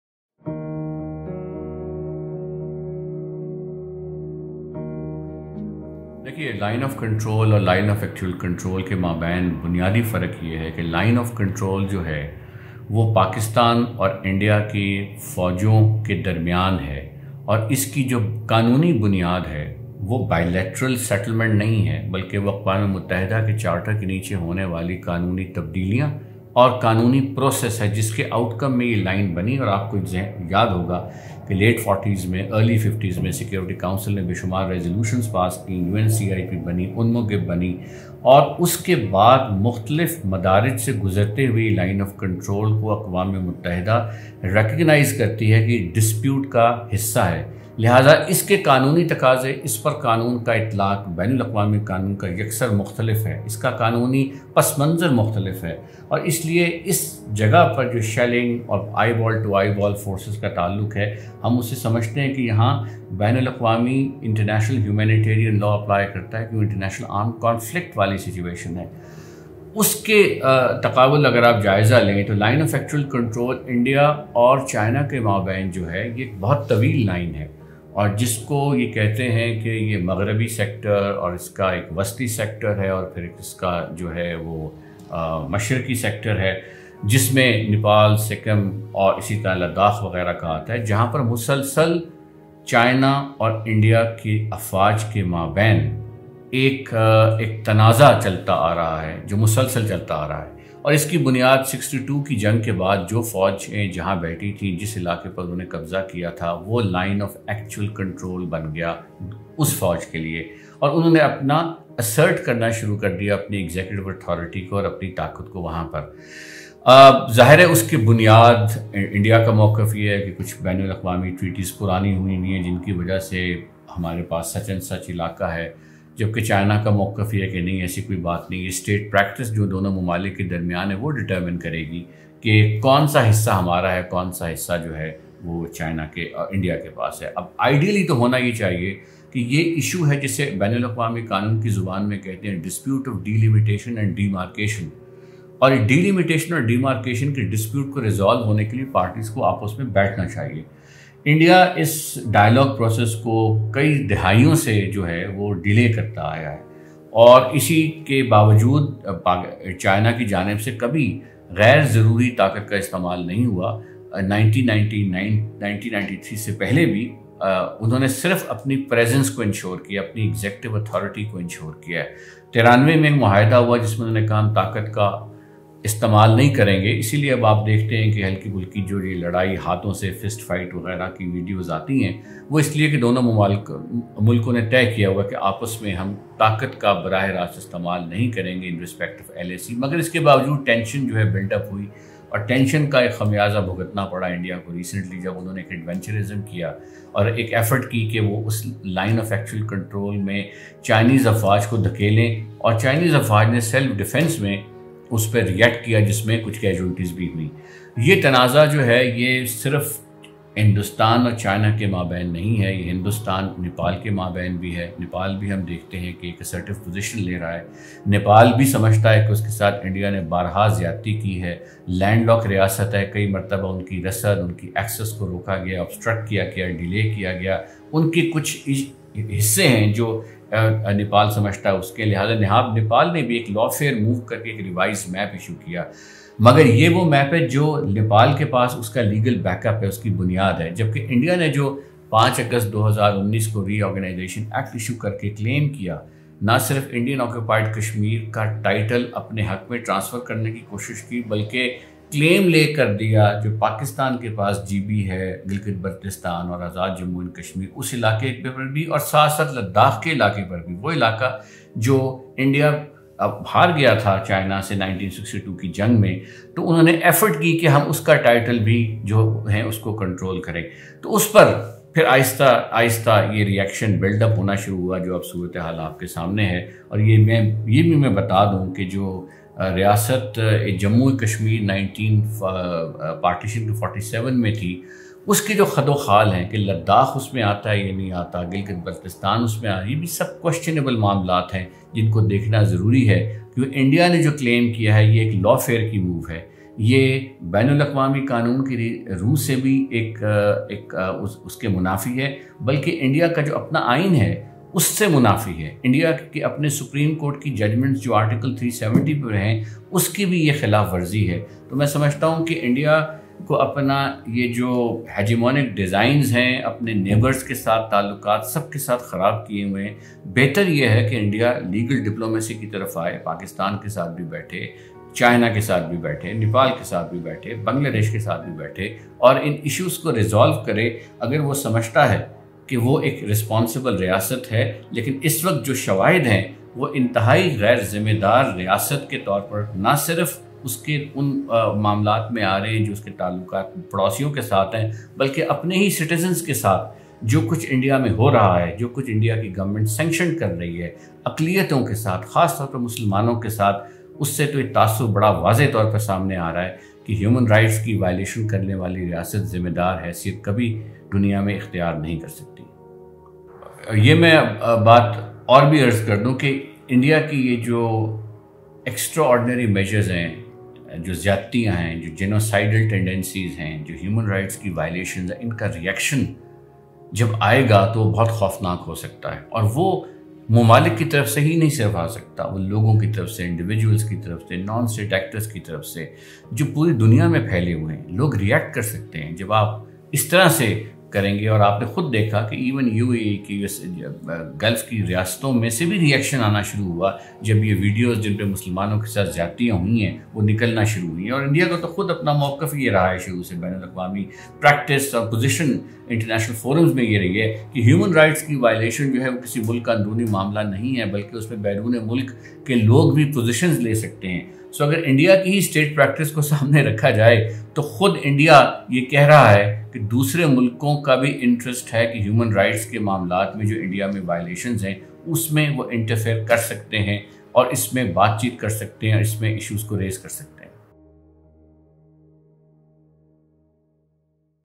The Situation in Ladakh - Analysis by Mr. Ahmer Bilal Soofi
Mr. Ahmer Bilal Soofi, President Research Society of International Law RSIL, analyses the current situation in Ladakh and the tensions between India and China from a legal perspective, while assessing its impact on neighboring countries including Nepal and Pakistan.